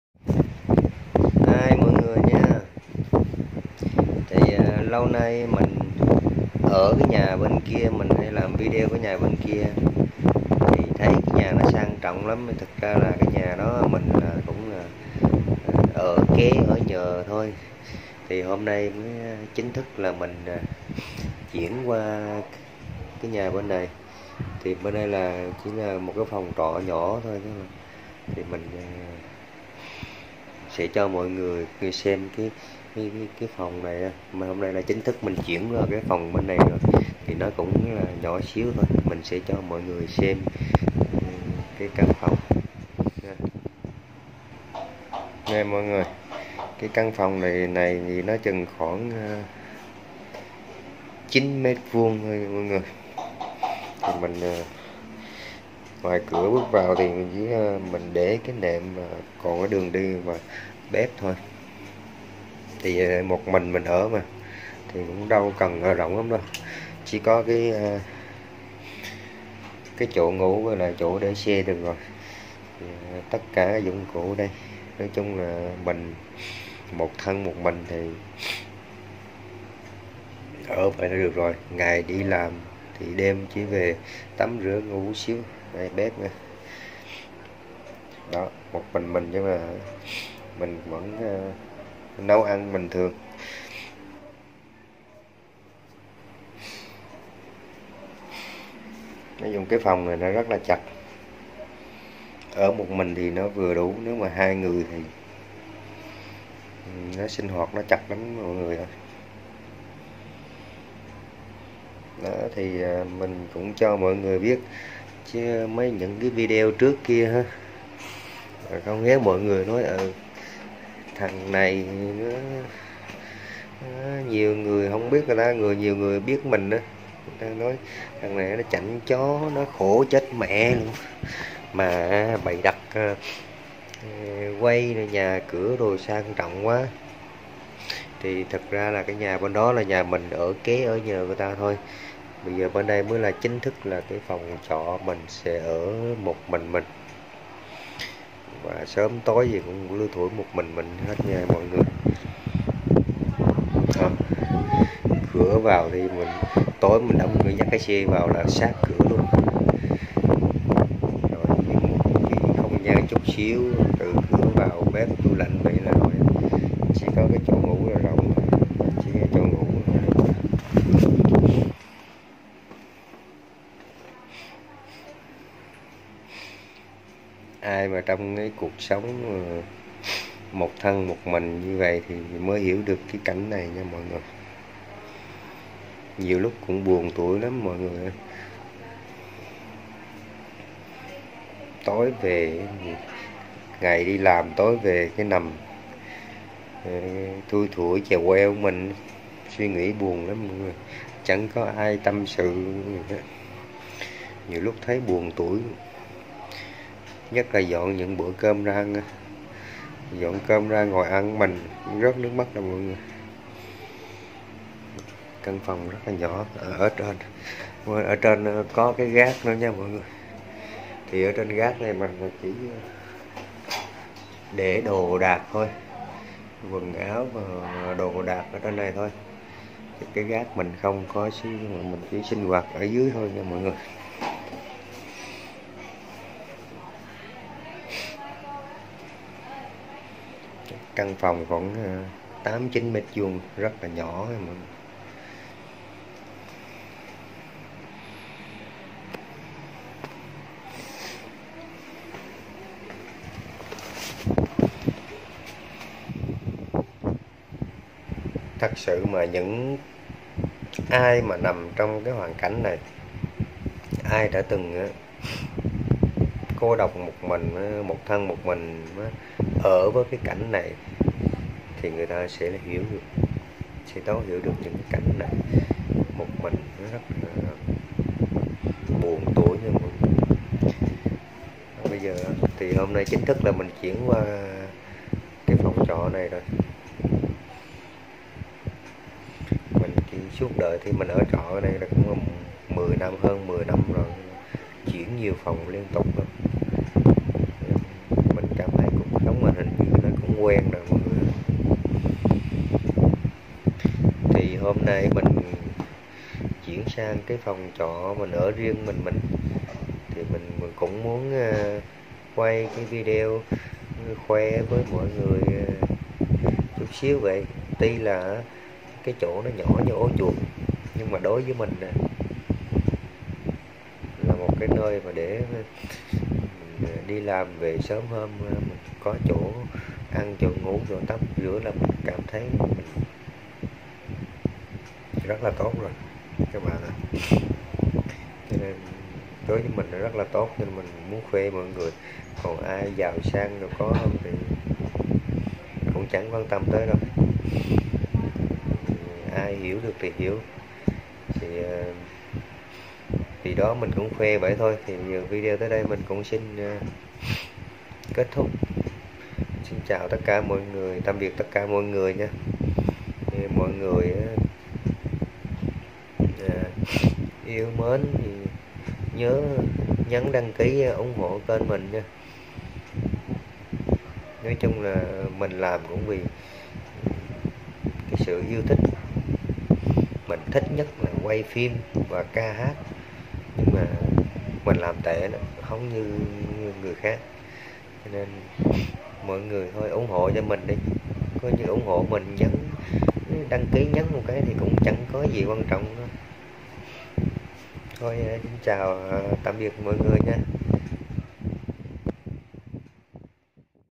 Ai xem video mà ko rớt nước mắt vì cảm động mới lạ/Hải Đăng Vlog
Đây là phòng trọ đơn sơ mà mình chính thức chuyển về ở một mình cô độc.
Cảm ơn tất cả mọi người đặc biệt những ai đã xem video.